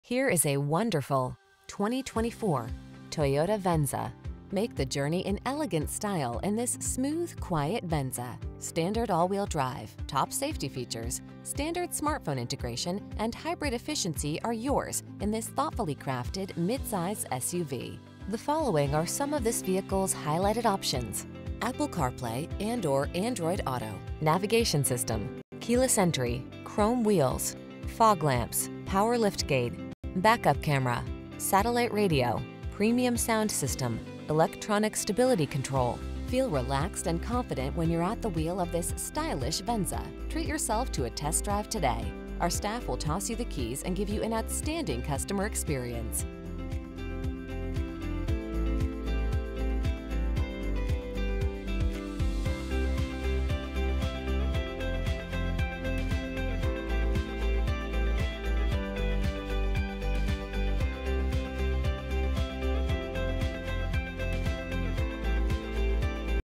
2024 Toyota Venza North Chicago, Oak Lawn, Calumet city, Orland Park, Matteson, IL 24349
New 2024 Toyota Venza XLE available in Chicago, Illinois at Toyota on Western. Servicing the Oak Lawn, Calumet city, Orland Park, Matteson, IL area.

6941 South Western Ave.

The 2024 Toyota Venza is an elegant and sophisticated hybrid SUV that combines comfort with advanced technology. Manufactured in Toyota City Japan this vehicle is designed to offer a smooth and efficient driving experience featuring a 2.5-liter engine paired with an electric motor resulting in a combined output of 176 horsepower. The Venza provides ample space and luxury amenities including an all-wheel-drive system electronic continuously variable transmission (e-CVT) and a host of safety features like adaptive cruise control lane departure warning and an automatic pedestrian alerting system.brbr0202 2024 Toyota Venza AWD Continuously Variable (ECVT) 2.5L 4-Cylinder DOHC 16V AWD.brbr40/37 City/Highway MPG